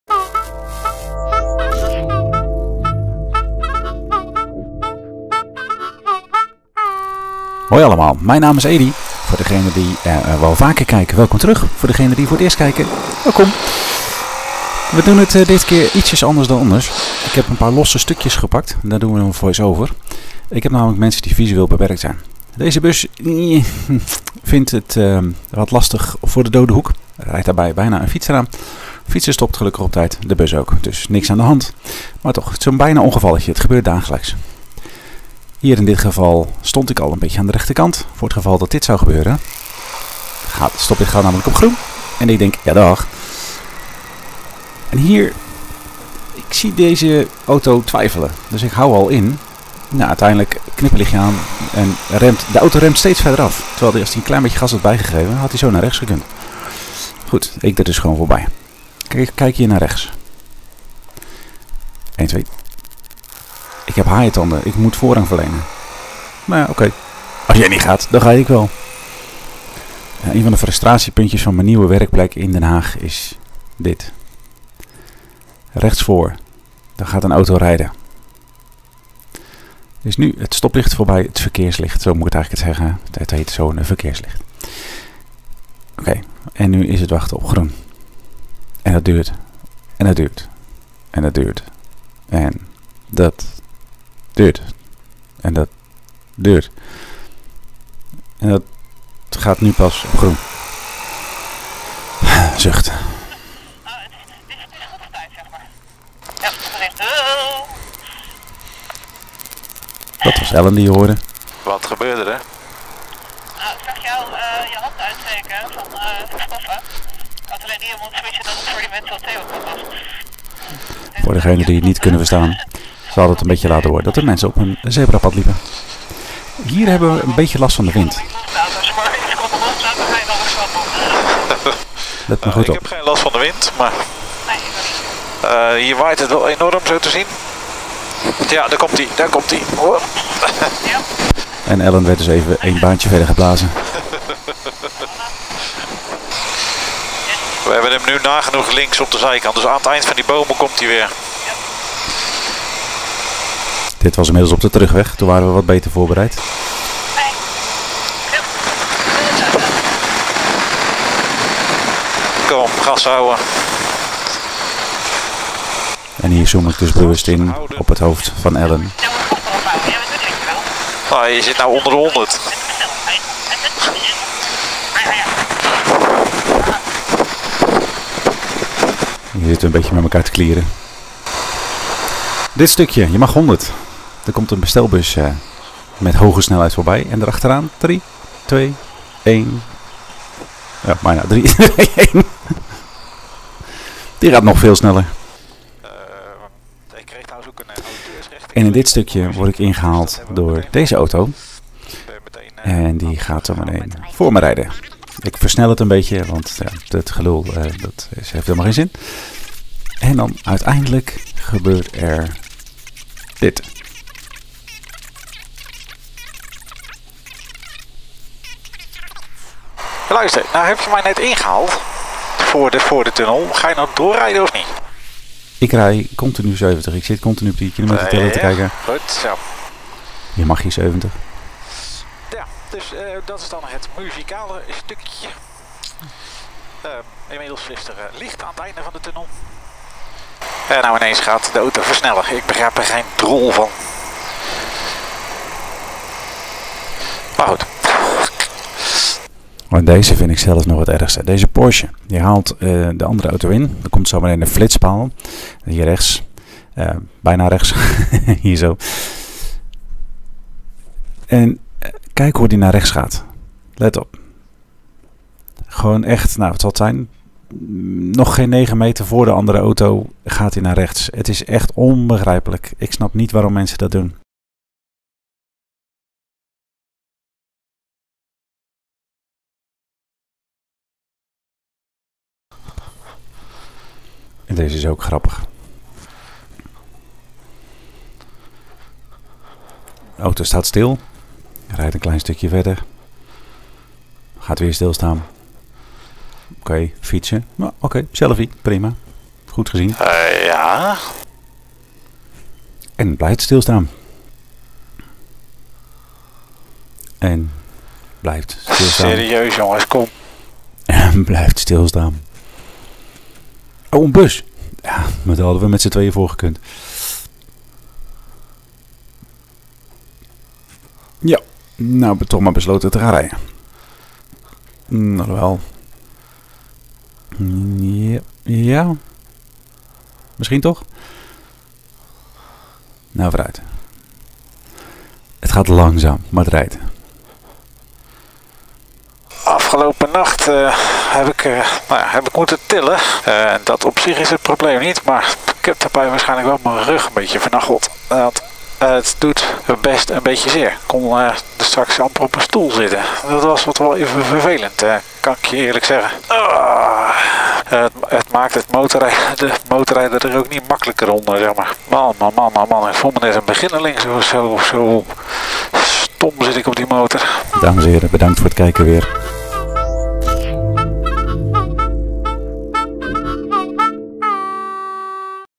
Frustratiemomentjes en bijzondere verkeerssituaties.
Vandaag even een klein beetje anders dan normaal. Ik heb een aantal beelden bij elkaar gezocht die opvielen en ik wel grappig vond om te delen.
Ik doe het op deze manier, zodat ook mijn volgers met een visuele handicap het hopelijk kunnen volgen.

Mijn dagelijkse motorritten

Gefilmd met:
Hoofdcamera
Sony FDR-X1000-VR

Motor
- Honda ST1300 Pan European 2004
- Dagrijverlichting in de richtingaanwijzers
- V-stream ruit
- TomTom 410 navigatie
- Autocom
- Mobilifoon